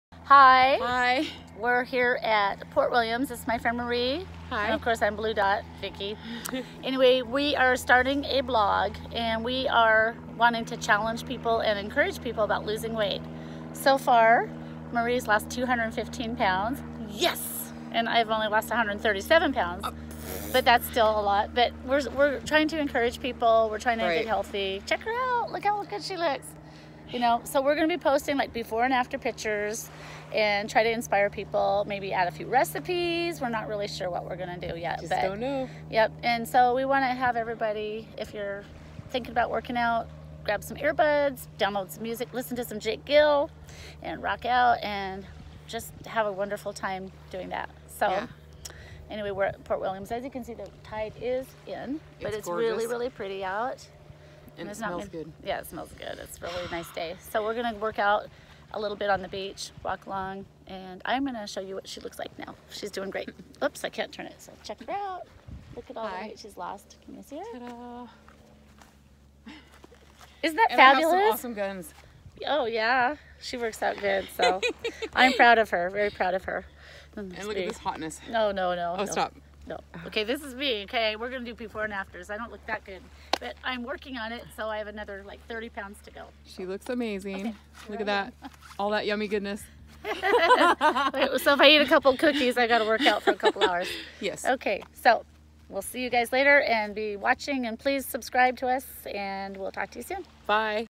Bluedots Adventures WEIGHT LOSS
We want to challenge and inspire people to lose weight by cutting your sugars, carbs & diet sodas from your diet. Drink plenty of water & get some good exercise.  Also surround yourself with positive people who encourage you in life, not try and bring you down. 🔵🏋🏻‍♀️😊🌞.